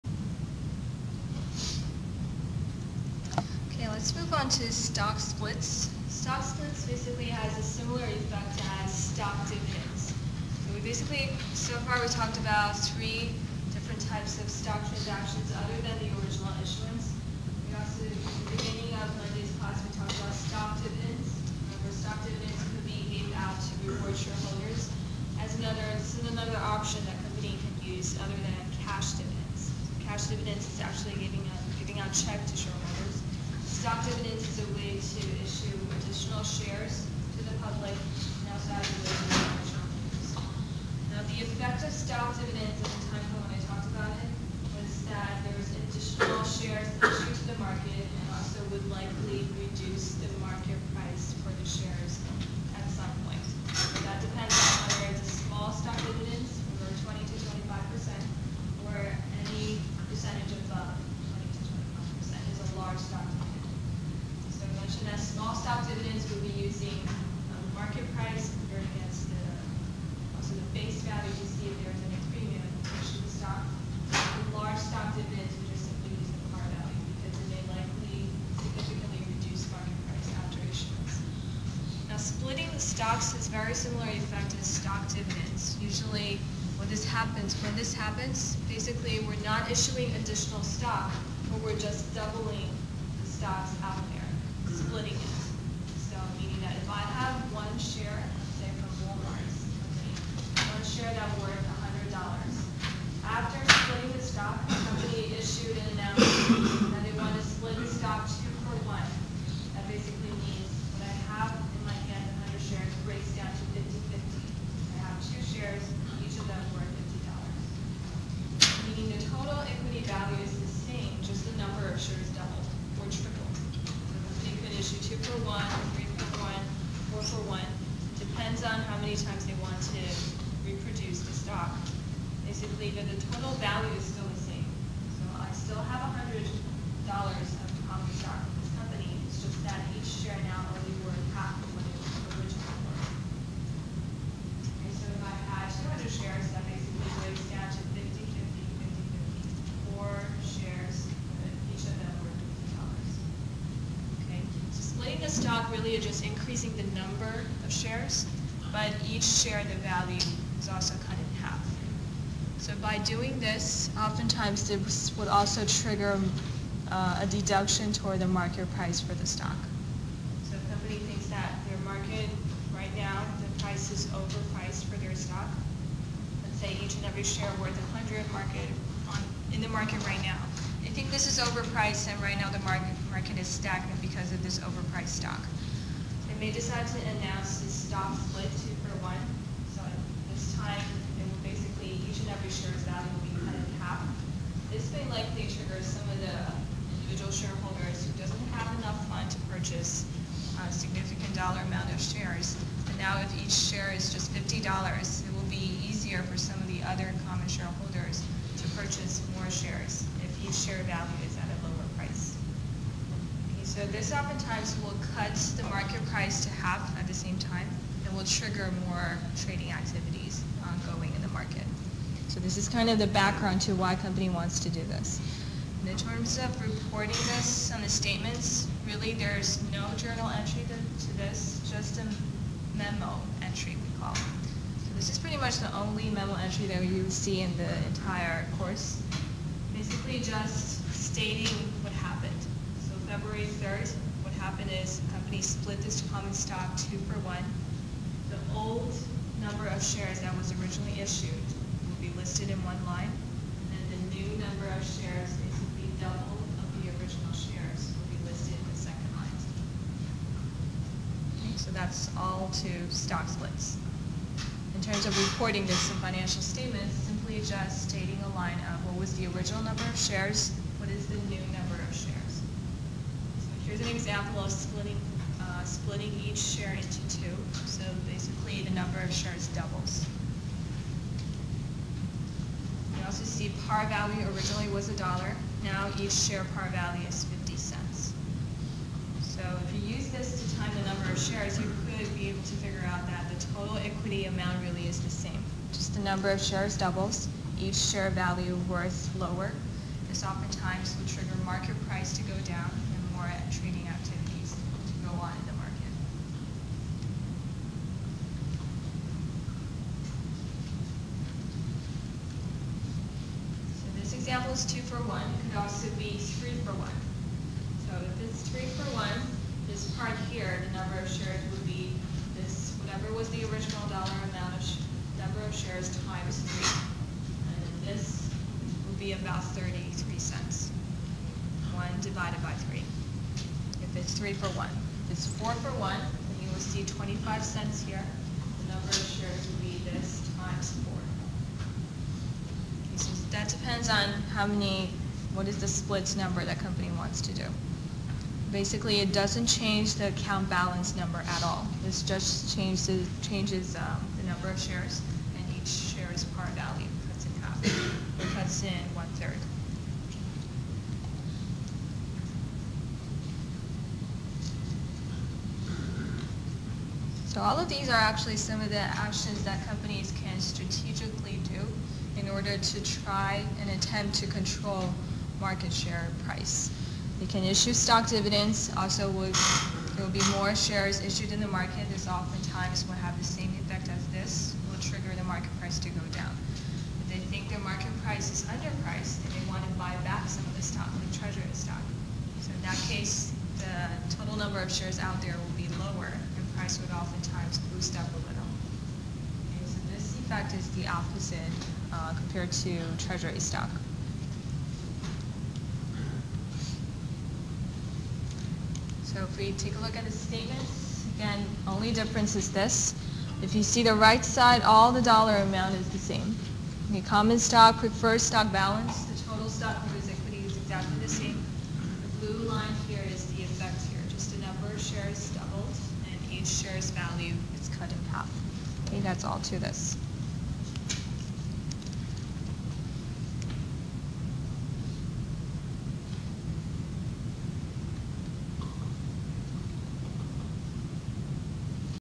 Stock Splits
Introduction to Financial Accounting

0:01 Accounting for the purchase and sale of treasury stock
0:12 Account for stock splits